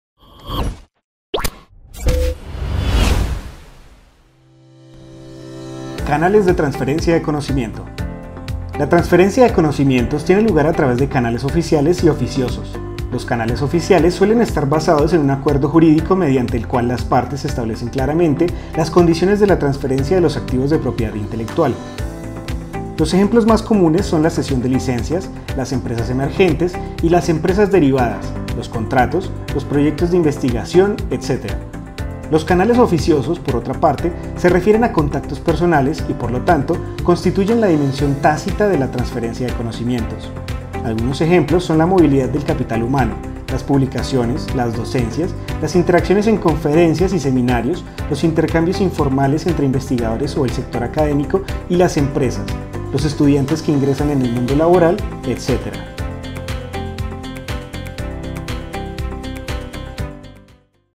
Canales de transferencia de conocimiento
La transferencia de conocimientos tiene lugar a través de canales oficiales y oficiosos. Los canales oficiales suelen estar basados en un acuerdo jurídico mediante el cual las partes establecen claramente las condiciones de la transferencia de los activos de propiedad intelectual.